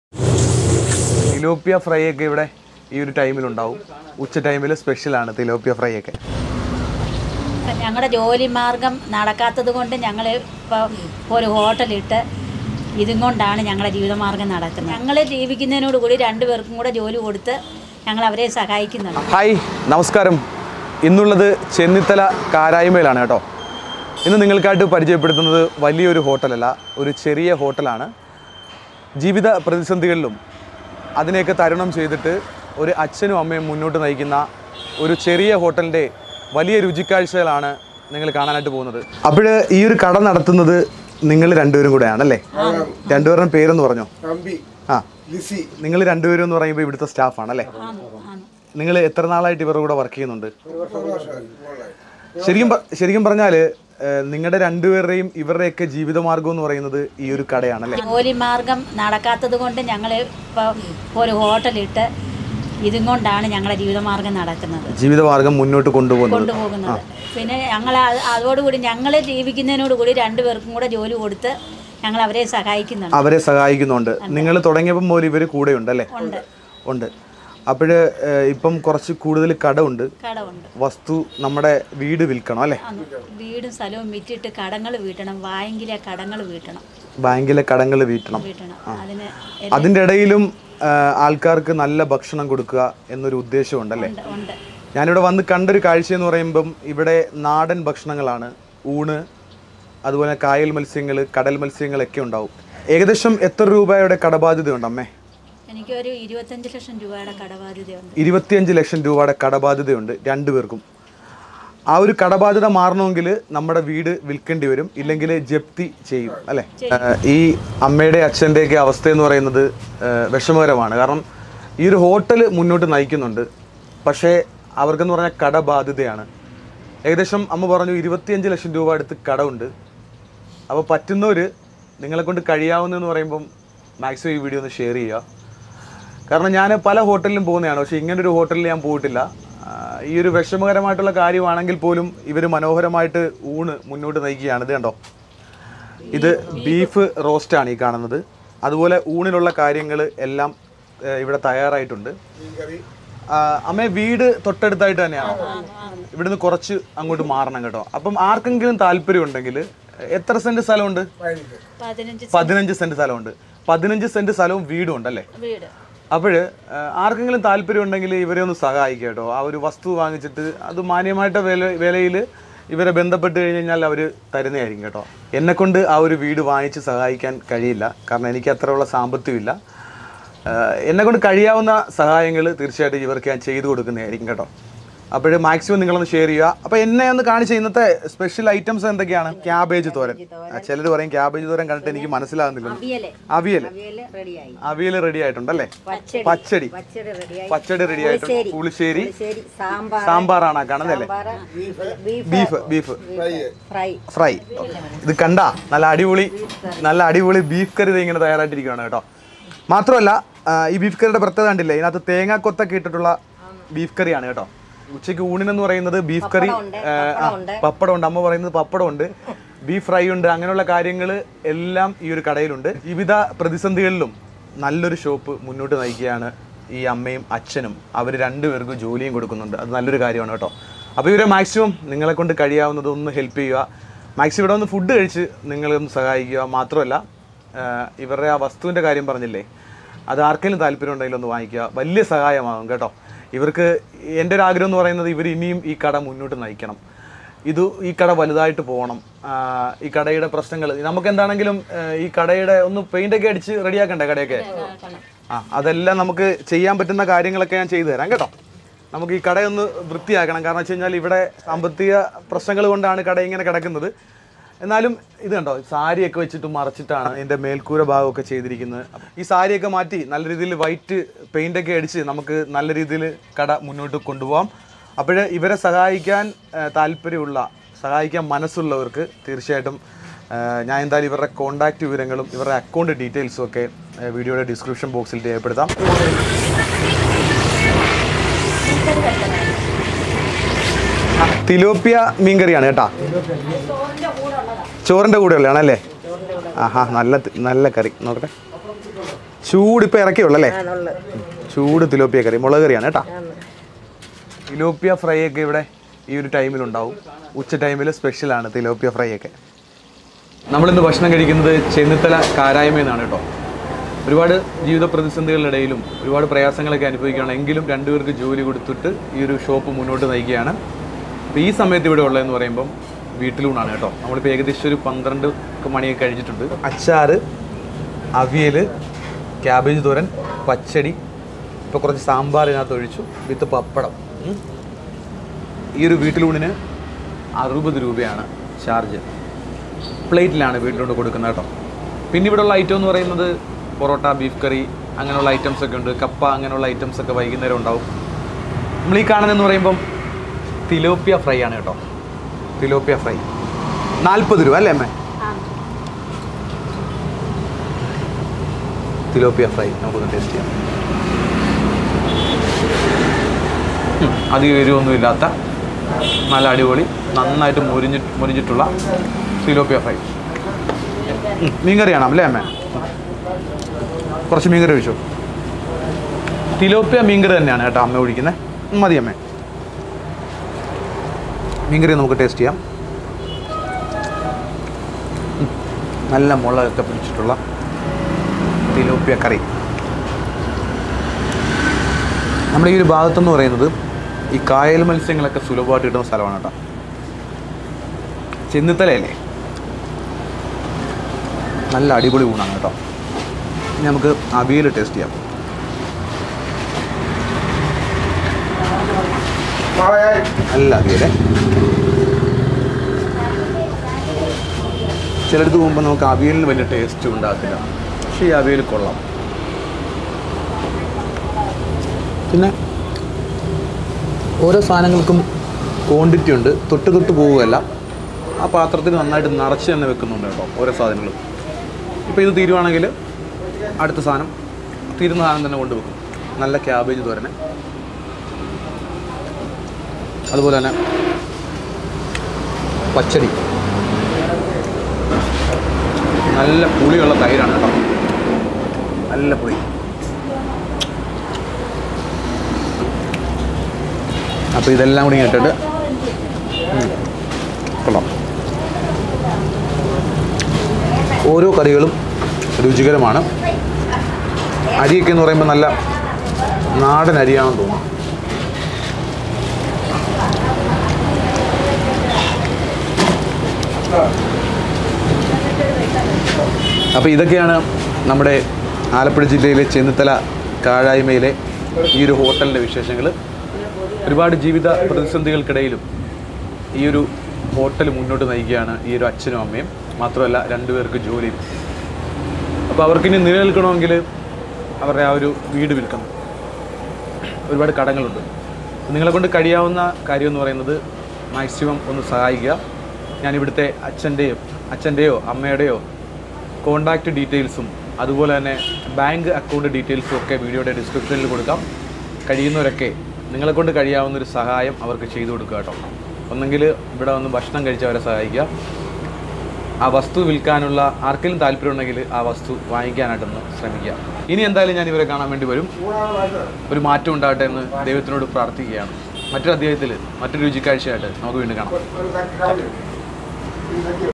10 കറികളും പുഴമീൻ വറുത്തതും ചെന്നിത്തലയിലെ ഈ ഹോട്ടൽ 4 പേരുടെ ജീവിതമാണ് | Street Food Kerala
10 കറികളും പുഴമീൻ വറുത്തതും ചെന്നിത്തലയിലെ ഈ ഹോട്ടൽ 4 പേരുടെ ജീവിതമാണ് | Street Food Kerala

MS SOSAMMA
Cherukole Branch

Location 👇
Karazhma Junction